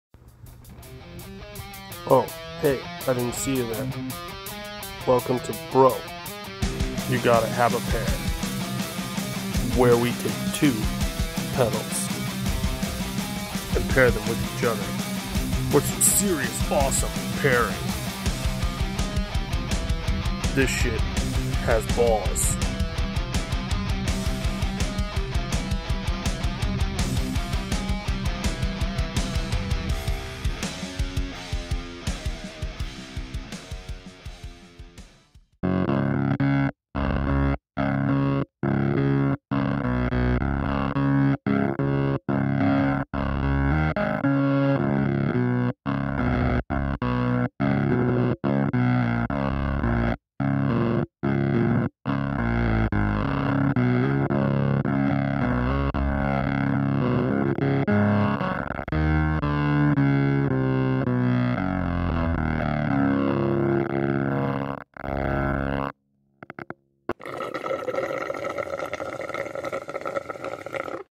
Bro, You Gotta Have a PAIR || Fukt Fuzz & Ventura Vibe
BRO! Get a pair! Potent pairings of SERIOUSLY AWESOME pedals with Chad von Douchington III.
TODAY - Demonic Machines Fukt Fuzz & DigiTech Ventura Vibe

‼️ Be sure to check out our generous sponsors and show them some love! ‼️